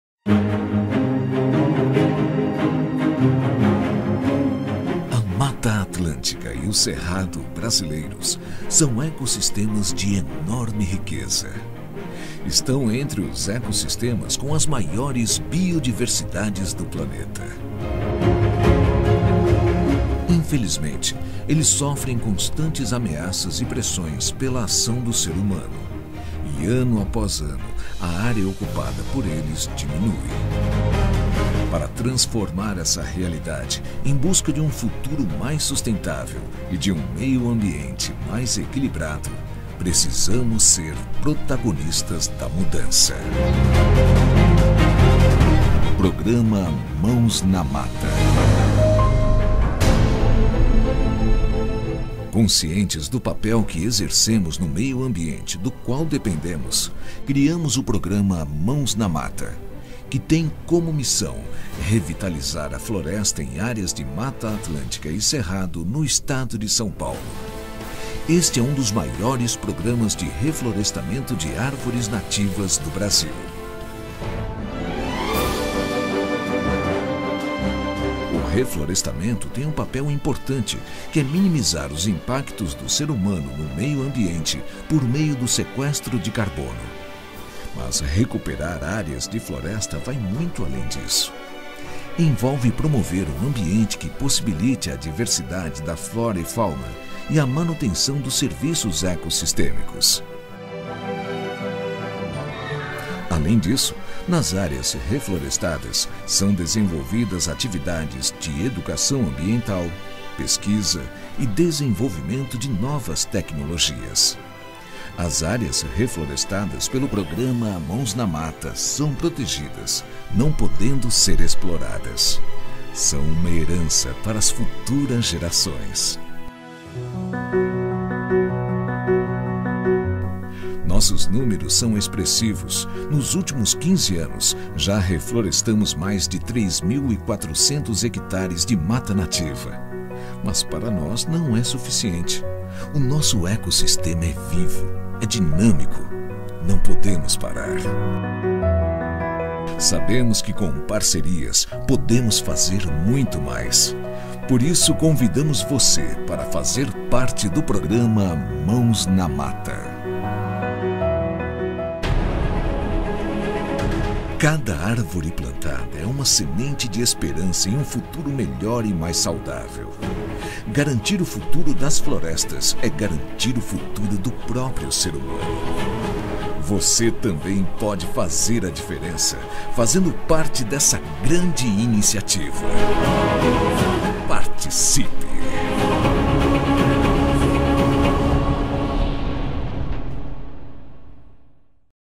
Mãos na Mata da AES Brasil na Voz de César Willian
Belíssimo trabalho, especialmente, se como eu, você aprecia a Natureza e os programas que trabalham por sua preservação e recuperação, o caso de Mãos na Mata da AES Eletropualo, agora, AES Brasil. Muito obrigado. Graças a DEUS.